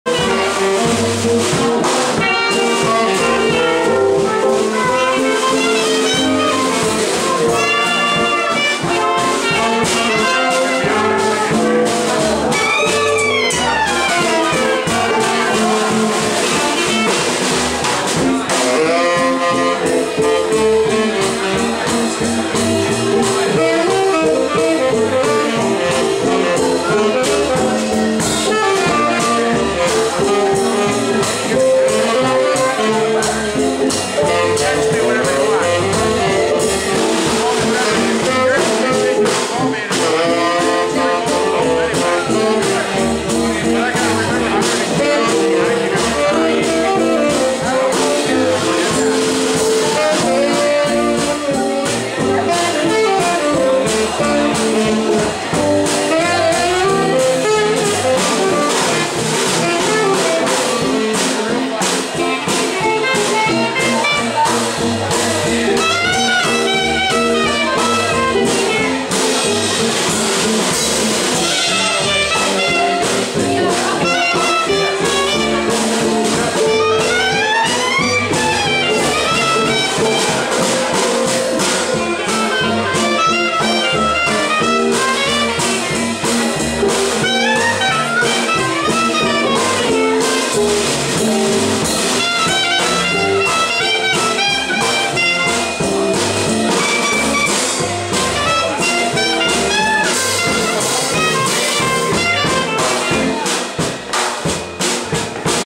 Lionel Ferbos and His Louisiana Shakers
Lionel Ferbos, the oldest still active New Orleans jazz musician (he's 99 here!) plays with his Louisiana Shakers band at a Nola Jitterbugs dance party. Desperado's on Frenchmen in New Orleans, August 2010.